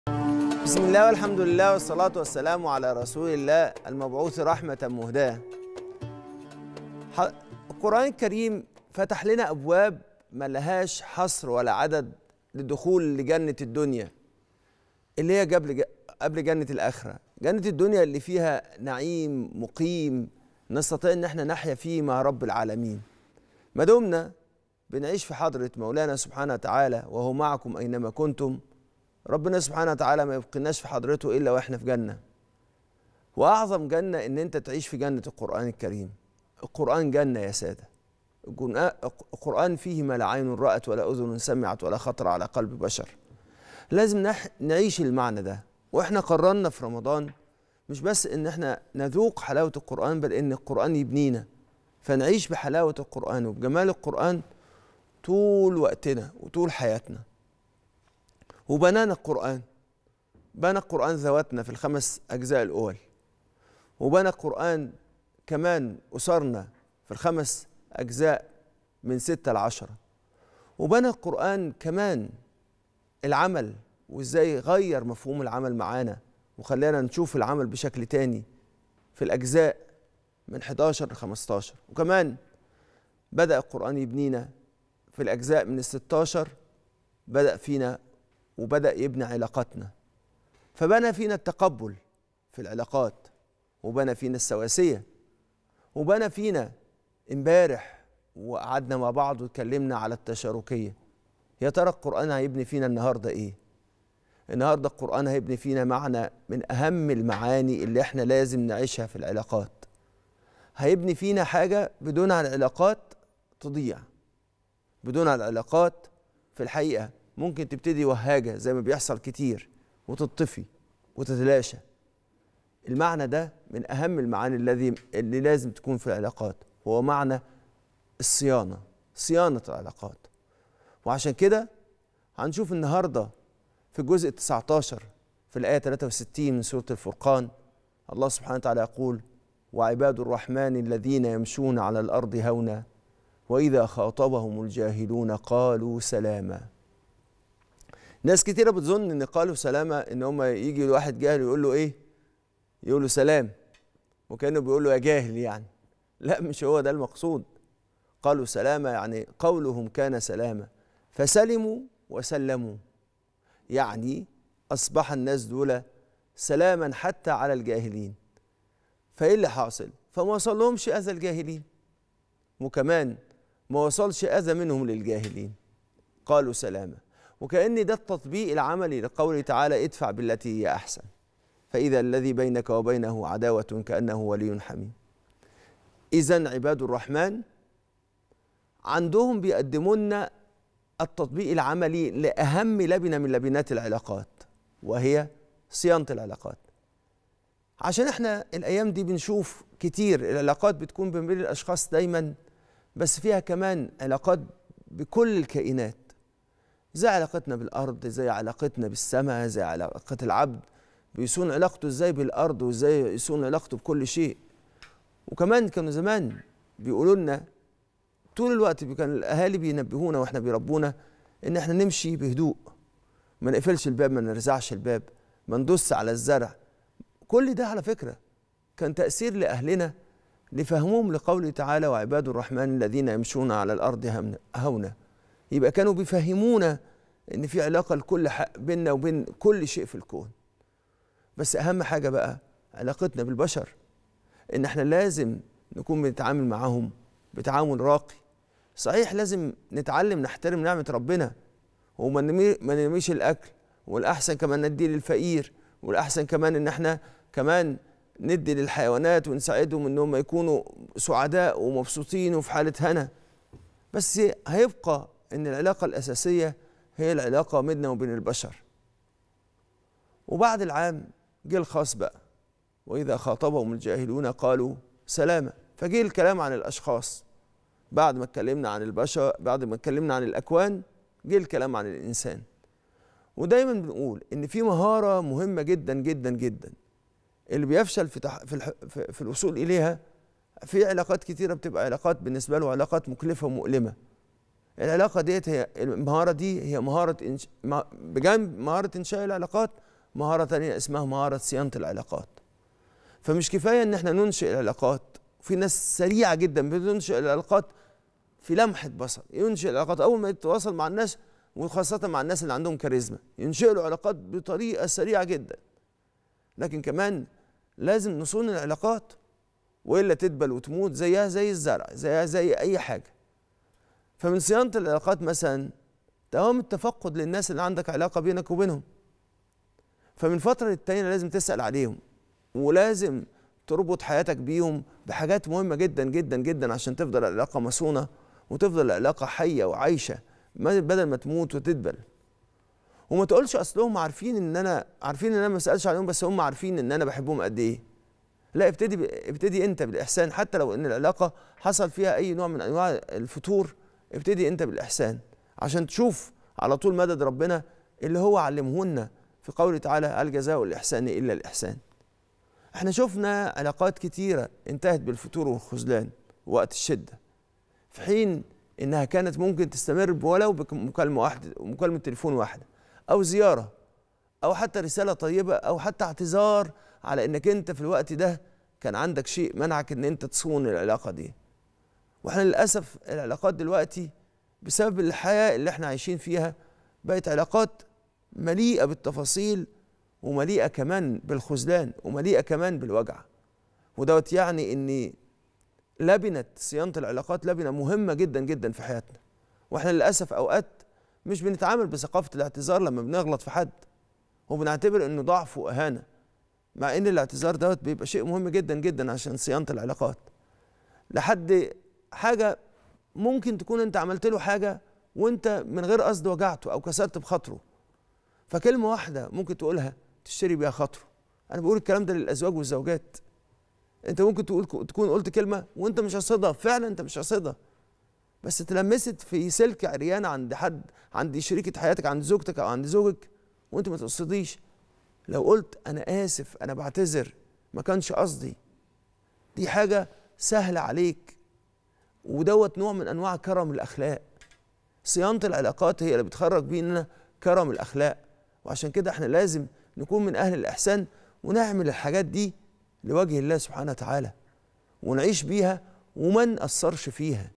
"وَإِذَا خَاطَبَهُمُ الْجَاهِلُونَ قَالُوا سَلامًا"د. عمرو الورداني: يكشف أهمية صيانة علاقتنا بالبشر
ولا تعسروا | " وَإِذَا خَاطَبَهُمُ الْجَاهِلُونَ قَالُوا سَلامًا" د. عمرو الورداني: يكشف دلالات الأية على أهمية صيانة علاقتنا بالبشر